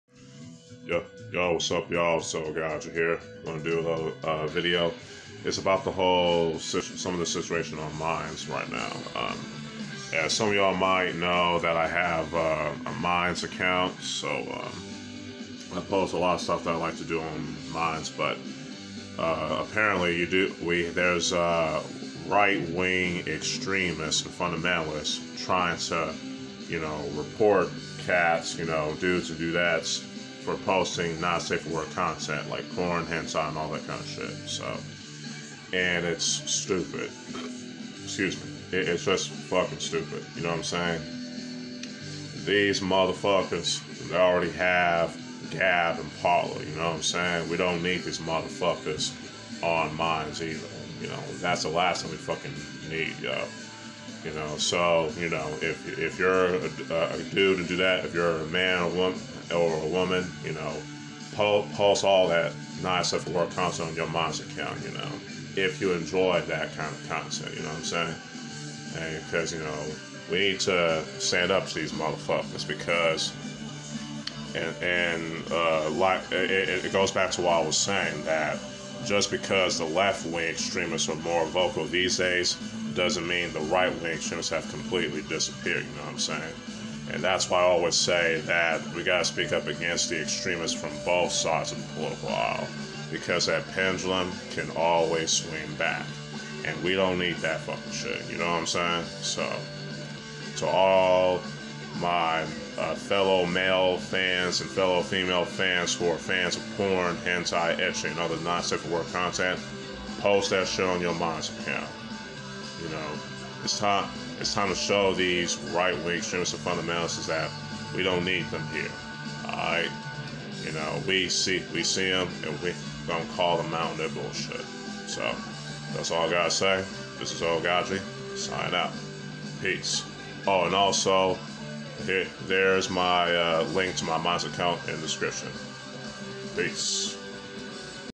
Oaghaji Rants: Right Wing Extremists reportin' dudes and dudettes on Minds
My rant on right wing extremists reportin' dudes and dudettes on Minds for postin' NSFW content
My link to my Minds account